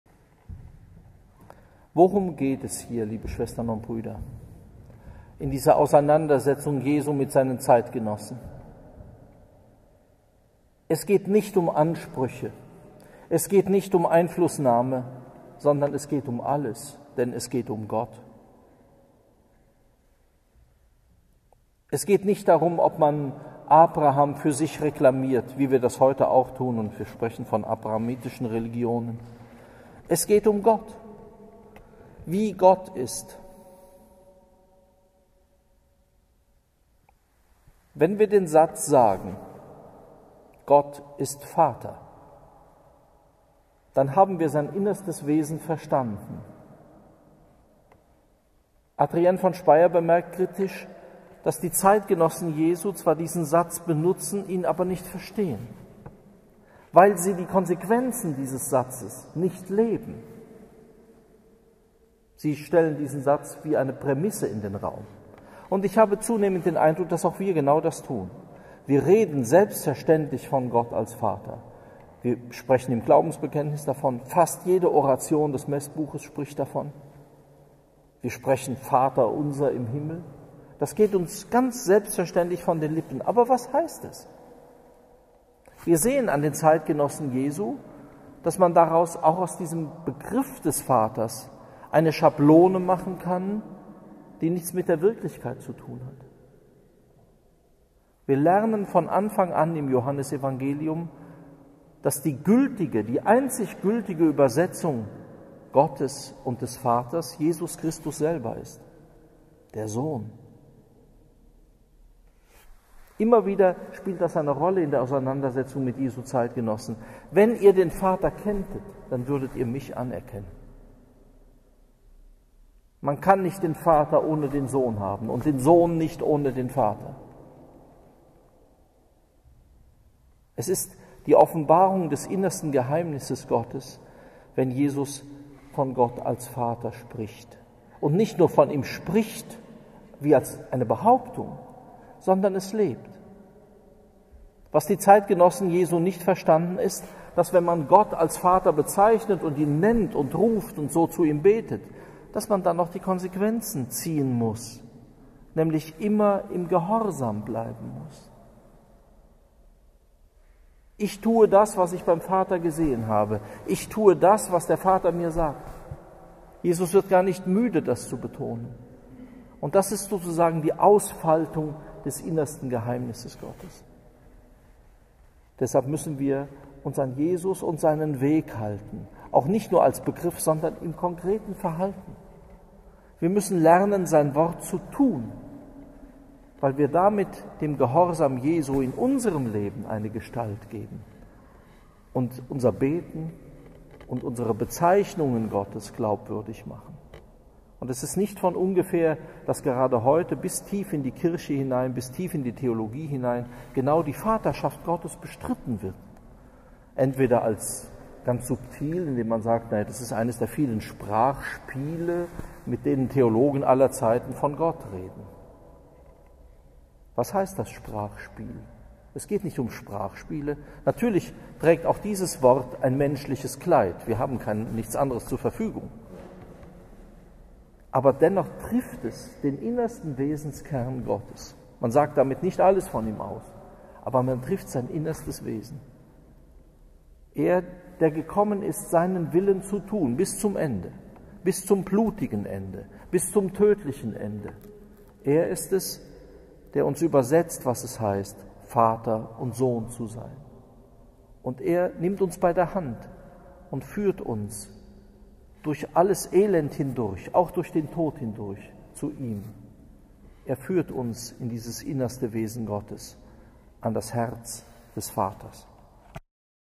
Es geht um alles, denn es geht um Gott!
Predigt am Do der 5.Fastenwoche im Kloster am Rande der Stadt/SB mit Rektor Dr.Johannes J.Kreier.